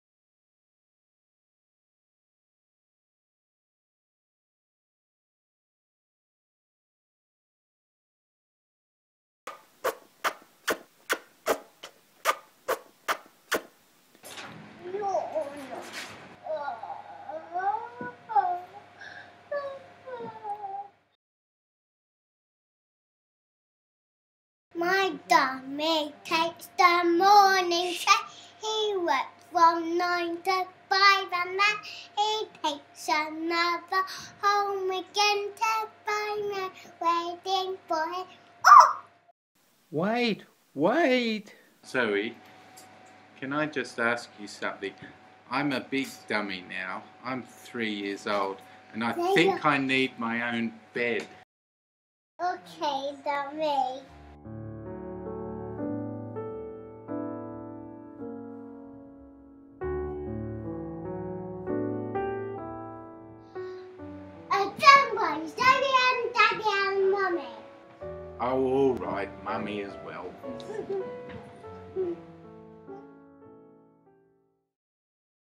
My dummy
Animation made by a parent and child during a Lambeth CLC Family Learning Animation course at Little Starz, Lambeth. Tutor: Marie Lenclos. 2011